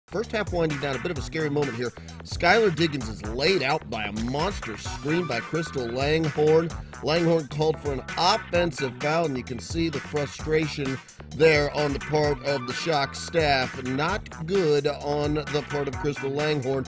Skylar Diggins Wiped Out By Crystal Langhorne Screen
WNBA.com